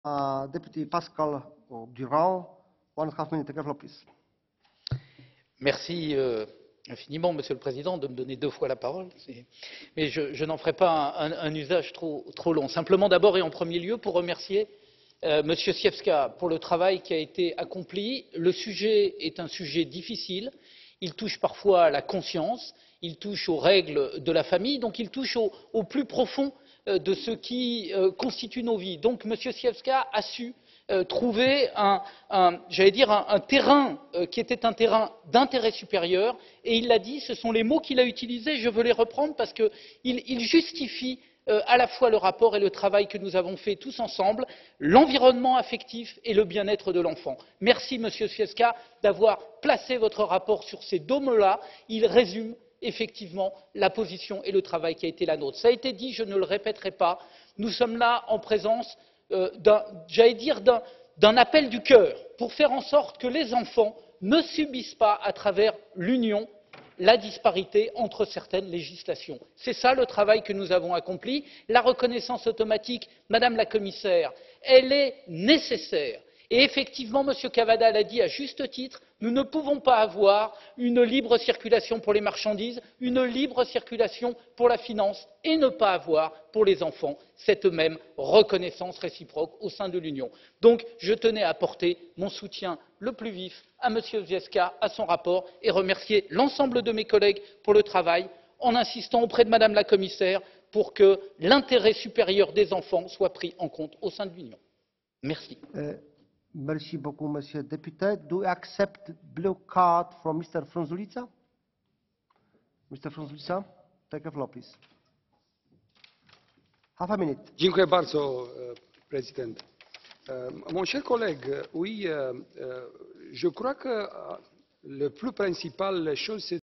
Adoption dans l'UE : permettre la libre circulation de toutes les familles
Pascal Durand intervient à l'occasion d'un débat sur un rapport traitant des aspects transfrontaliers des adoptions.
Ce rapport propose notamment des réponses pour palier à la non-reconnaissance problématique de certaines adoptions d'un pays de l'UE à l'autre, notamment pour les couples de même sexe.